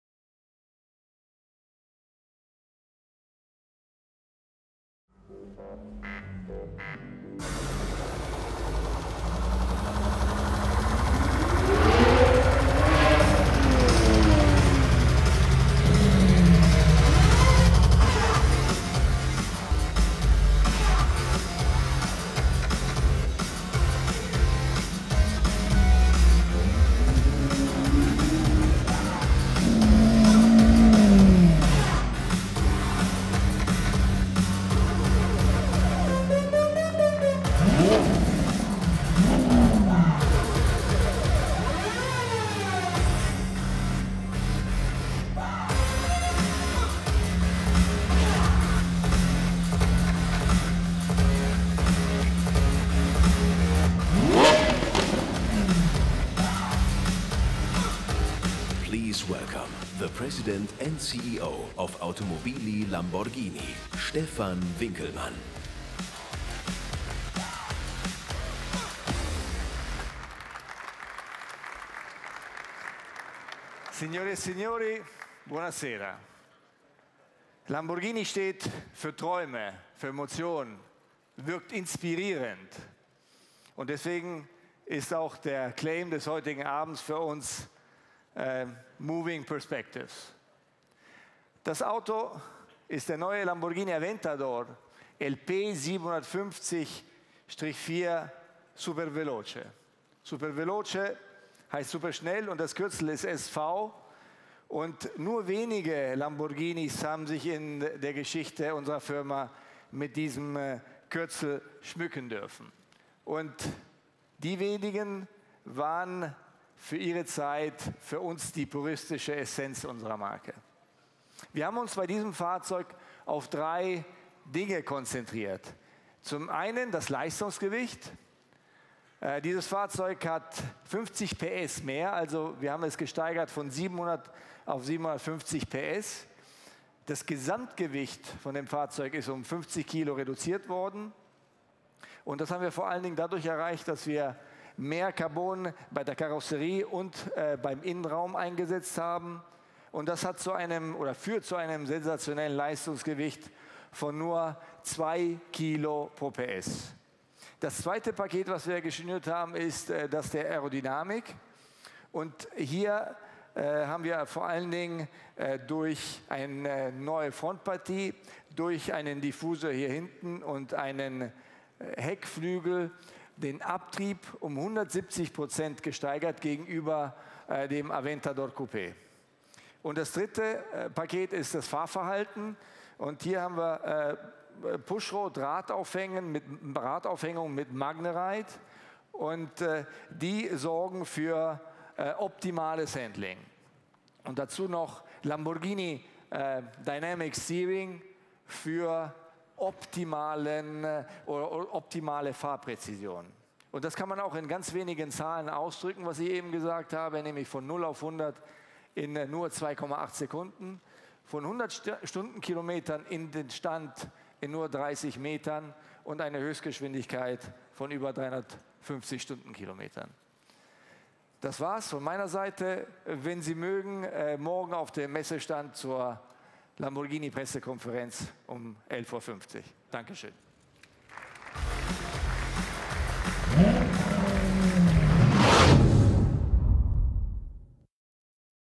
Lamborghini Aventador LP 750-4 Superveloce - Worldwide premiere
Just as you would expect from an SV model, the more hardcore Aventador comes with additional power in a lighter package as it has gained an extra 50 HP to 750 HP (552 kW) while losing 50 kg (now at 1,525 kg) thanks to a more extensive use of carbon fiber. It has enabled the supercar to achieve a 0-62 mph (0-100 km/h) sprint in just 2.8 seconds (0.1s quicker) before topping out at more than 217 mph (350 km/h).